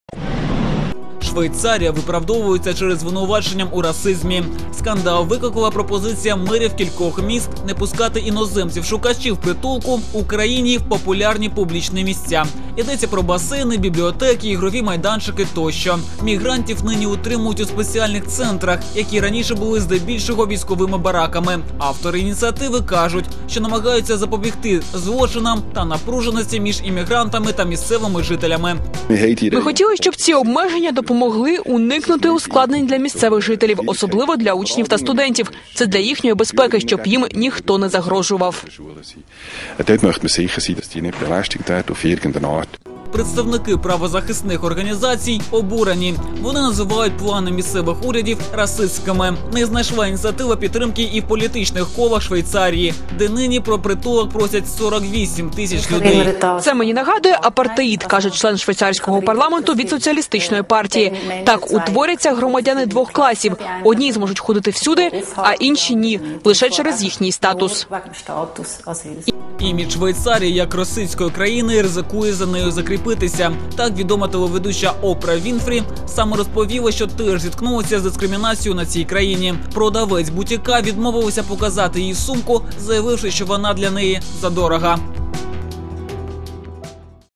Швейцарию обвиняют в расизме - заслуженно?
Швейцария не согласна с обвинениями в расизме. Причина скандала - предложение мэров нескольких городов не пускать иностранцев-искателей убежища в популярные публичные места. Речь идет о бассейнах, библиотеках, игровых площадках и т.д.. Мигрантов сейчас удерживают в специальных центрах, которые ранее использовались как военные казармы.
Авторы инициативы говорят, что пытаются предотвратить преступления и напряженности между иммигрантами и местными жителями. Представители правозащитных организаций - возмущены и называют планы местных правительств расистскими. Не нашла инициатива поддержки и в политических кругах Швейцарии.
Между тем имидж Швейцарии портит еще один расистский скандал. Известная телеведущая Опра Уинфри недавно рассказала, что тоже столкнулась с дискриминацией в этой стране. Продавец бутика отказалась показать ей сумку, заявив, что эта сумка для нее слишком дорогая. Подробности -- в сюжете журналистов телеканала 100% NEWS. Смотрите прямо сейчас!

КОМПЛЕКСНАЯ РЕКЛАМА? ЛЕГКО!